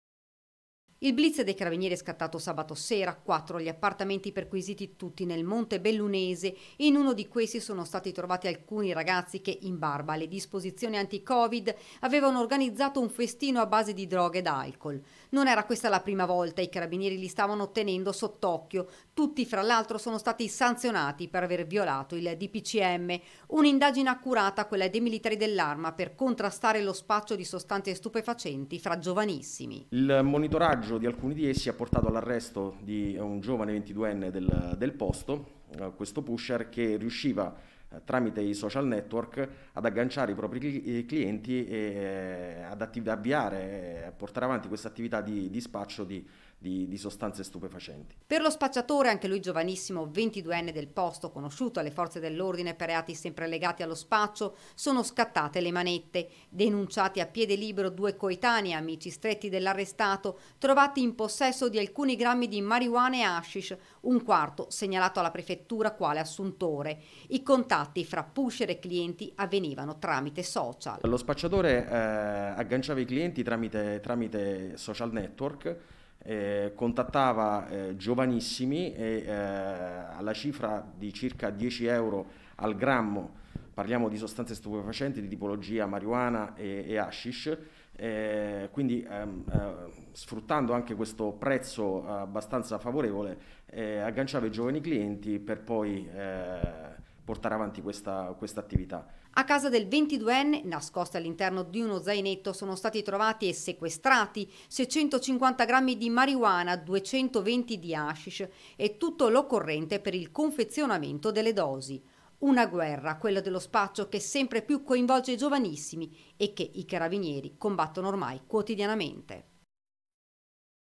FESTINI A BASE DI MARJUANA E HASHISH, ARRESTATO LO SPACCIATORE | 18/01/2021
A3 NEWS Treviso 18/01/2021 - MONTEBELLUNA - Festini a base di droga e alcol: a rifornire i giovani clienti uno spacciatore 22enne, arrestato dai carabinieri di Montebelluna || Il blitz dei carabinieri è scattato sabato sera : quattro gli appartamenti perquisiti, tutti nel montebellunese. In uno di questi, sono stati trovati alcuni ragazzi che in barba alle disposizioni anticovid avevano organizzato un festino a base di droga ed alcol. Non era questa la prima volta, i carabinieri li stavano tenendo sott'occhio. Tutti fra l'altro sono stati sanzionati per aver violato il dpcm. Un'indagine accurata quella dei militari dell'arma per contrastare lo spaccio di sostanze stupefacenti fra i giovanissimi, ''Il monitoraggio di alcuni ragazzi, ha portato all'arresto di un 22enne, pusher - spiega il Capitano Gabriele Favero, comandante della Compagnia dei carabinieri di Montebelluna che riusciva tramite o social ad agganciare i giovani clienti e a portare avanti questa attività di spaccio di sostanze stupefacenti''.Per lo spacciatore anche lui giovanissimo 22 anni del posto, conosciuto alle forze dell'ordine per reati sempre legati allo spaccio, sono scattate le manette. Denunciati a piede libero due coetanei amici stretti dell'arrestato trovati in possesso di alcuni grammi di marjuana e hashish, un quarto segnalato alla Prefettura quale assuntore.I contatti fra pusher e clienti avveniva tramite social'' Lo spacciatore, continua il Comandante Favero, agganciava i clienti tramite i social, contattava giovanissimi e vendeva lo stupefacente, alla cifra di 10 euro al grammo, e sfruttando questo prezzo favorevole, portava avanti questa attività''. A casa del 22enne nascosti all'interno di uno zainetto sono stati trovati e sequestrati 650 grammi di marjuana, 220 di hashish e tutto l'occorrente per il confezionamento delle dosi. Una guerra quella allo spaccio che sempre più coinvolge giovanissimi e che i carabinieri combattono ormai quotidianamente - Intervistati Cap. GABRIELE FAVERO (Comandante Compagnia Carabinieri Montebelluna ) (Servizio di Daniela Sitzia) - Segui Antenna Tre anche sul digitale terrestre! Visita il sito